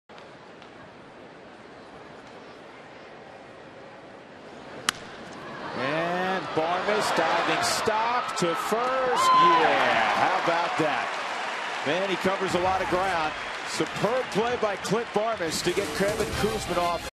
2009/05/30 Barmes' diving stop
SD@COL: Barmes' diving stop robs Kouzmanoff of a hit